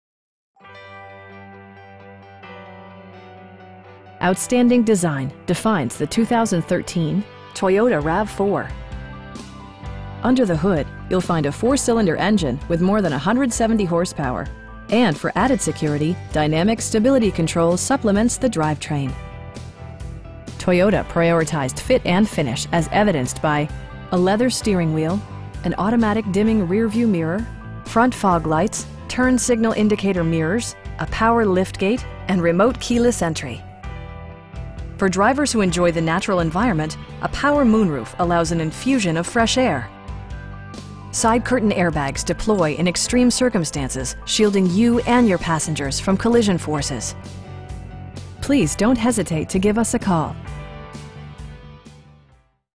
2013 Toyota RAV4 Limited in Dallas, TX 75228
Cowboy Toyota
9525 E. R L Thornton Freeway

Outstanding design defines the 2013 Toyota RAV4. Under the hood you'll find a 4 cylinder engine with more than 170 horsepower, and for added security, dynamic Stability Control supplements the drivetrain. Toyota prioritized fit and finish as evidenced by: a leather steering wheel, an automatic dimming rear-view mirror, front fog lights, turn signal indicator mirrors, a power liftgate, cruise control, and remote keyless entry. For drivers who enjoy the natural environment, a power moon roof allows an infusion of fresh air. Side curtain airbags deploy in extreme circumstances, shielding you and your passengers from collision forces. You will have a pleasant shopping experience that is fun, informative, and never high pressured. Please don't hesitate to give us a call.